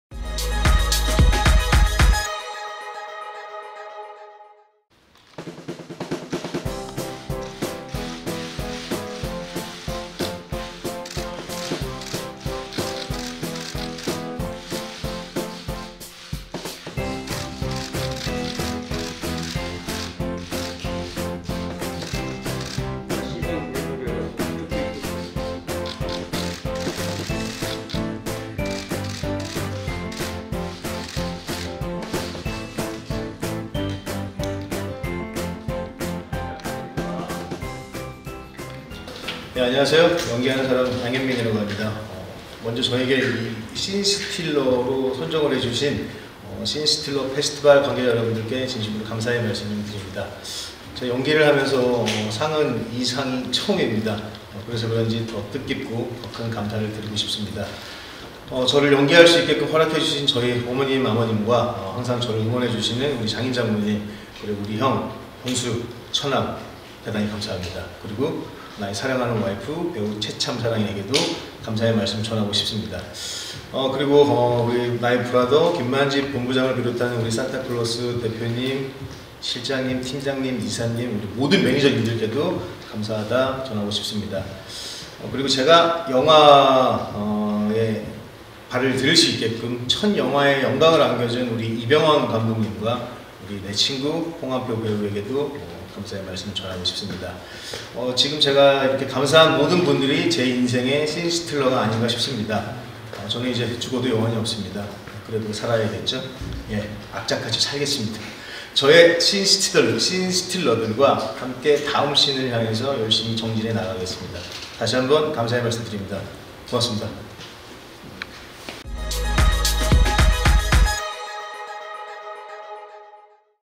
양현민 '사랑하는 배우 최참사랑에게 감사'
[티브이데일리 조혜인 기자] '신스틸러 페스티벌' 영화제가 7일 오후 서울 강남구 논현동 MIL STUDIO에서 열렸다.

이날 '신스틸러 페스티벌'에 배우 양현민이 참석해 자리를 빛냈다.

'신스틸러 페스티벌'은 영화나 TV 드라마 등에서 훌륭한 연기력이나 독특한 개성을 발휘해 주연 이상의 활약을 펼친 배우들을 위한 영화제다.